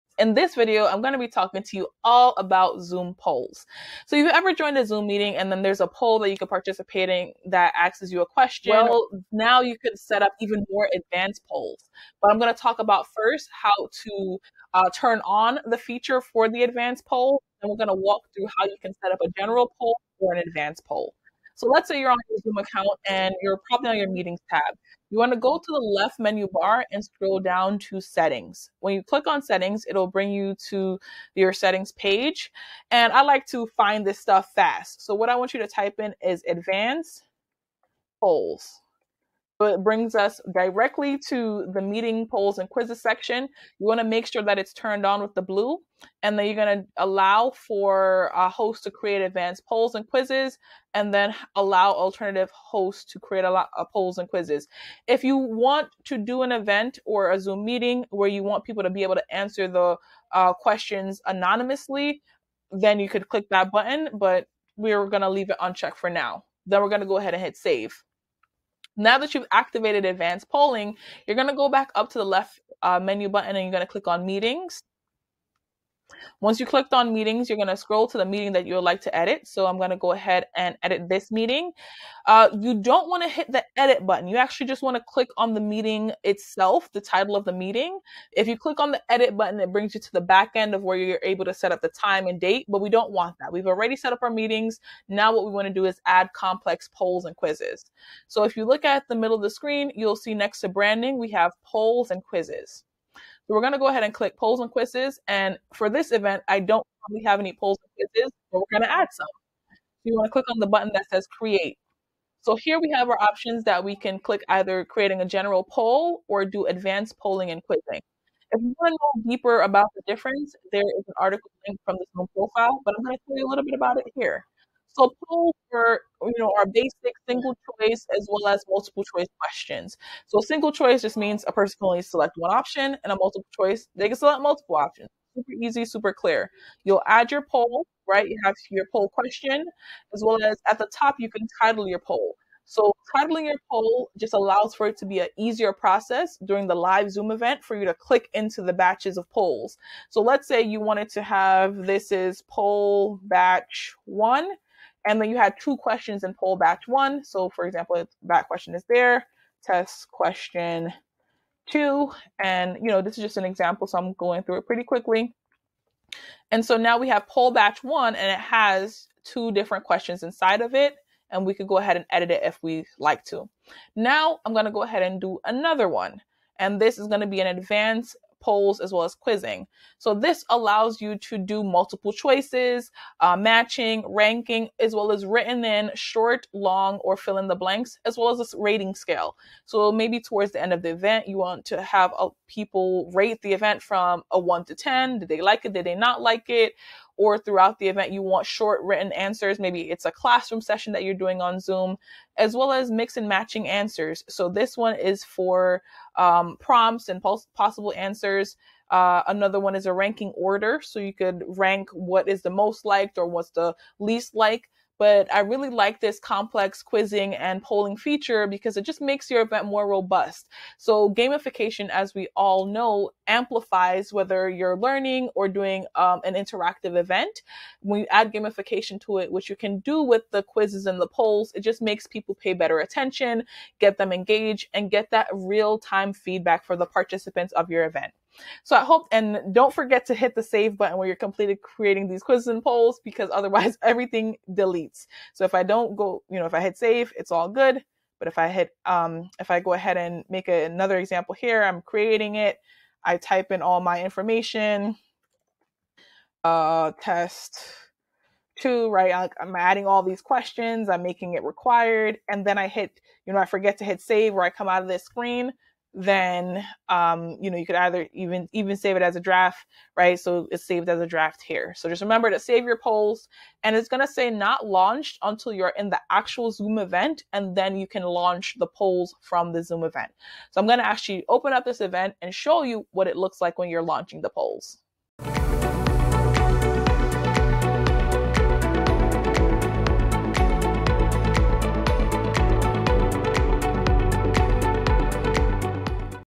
How to Create a Zoom Poll Before your Meeting
In this video, you’ll learn ALL the details on how to set up a Zoom poll for your meetings.

You won’t need to create it live, watch as Georgie-Ann Getton set up a poll prior to the event and how you can do it too.

Here are the steps:
1) Inside zoom.us/meeting, go to settings
2) Search for “advanced polls”
3) Allow host and alternative host to create polls and quizzes
4) Check the box if you want anonymous answers
5) Hit “Save”
6) Click on “Meetings”
7) Click on the meeting’s name (do NOT click on edit)
8) Click on polls/quizzes
9) Click create
10) Select polls or advanced polls
11) Choose single or multiple choice
12) Add your poll name, question, and options
13) Click “Save”

PART 2 - testing the polls - ON THE COMMENTS!

► If you’re ready to scale your business you're in the right place! We help small and medium businesses scale using digital tools.